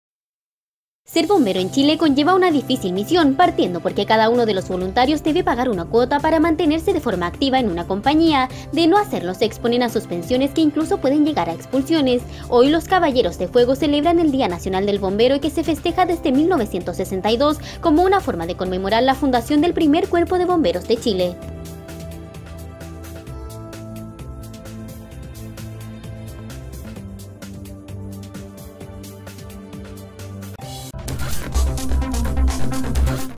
La difícil misión de ser un bombero voluntario en Chile
Deben pagar cuotas, arriesgar sus vidas en incendios, accidentes o catástrofes, e incluso exponerse a agresiones de la ciudadanía. Los “Caballeros del fuego” celebran hoy su día nacional y en Iquique lo festejaron junto a los niños.